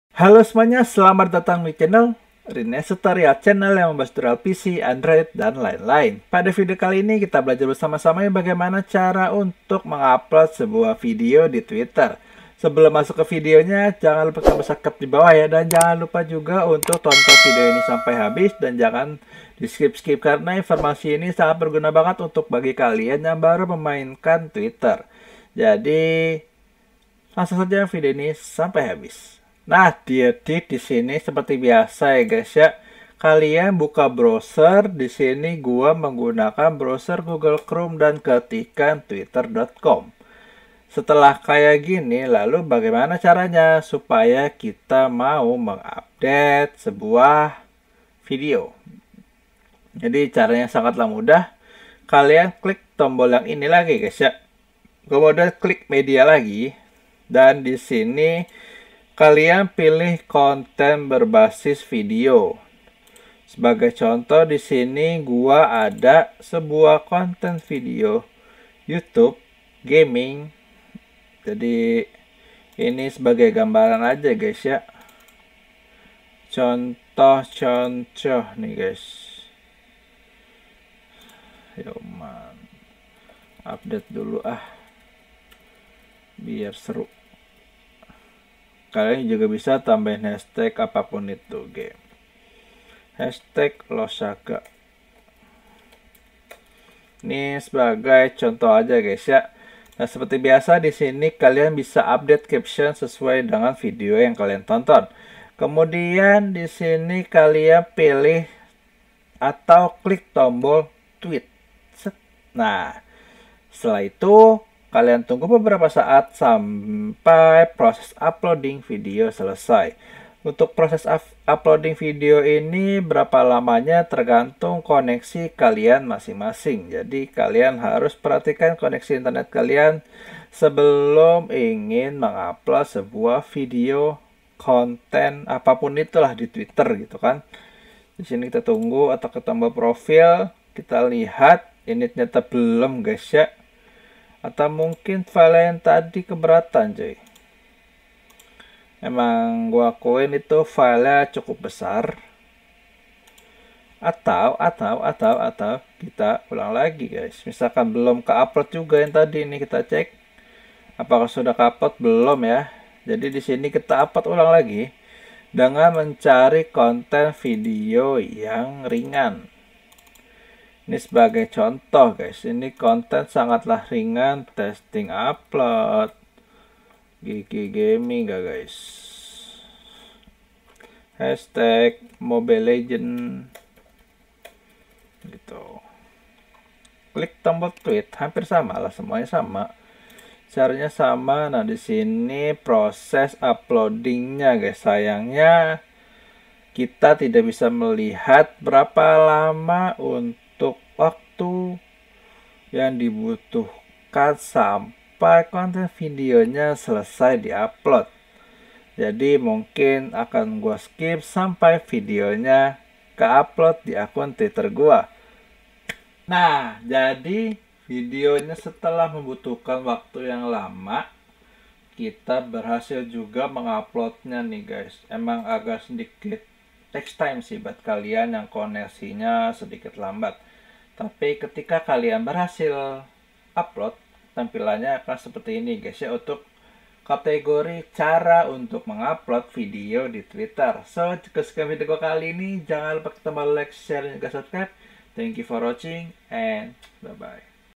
Cara Upload Video di Twitter Lewat PC Terbaru dan Simpel
Hai Semuanya,

Selamat datang di channel Rines Tutorials, Twitter tidak hanya digunakan untuk mencari pertemanan dan berita saja melainkan disini kamu bisa curhat sendiri dan membagikan moment keseruan kamu kepada orang banyak melalui video juga.

Dimana nantinya diharapkan video yang kamu tonton banyak disukai oleh para pemain twitter lainnya.

Di Video kali ini mari kita belajar bersama - sama yuk cara upload video di twitter lewat pc terbaru dan simpel banget untuk kamu coba sekarang.

Terima Kasih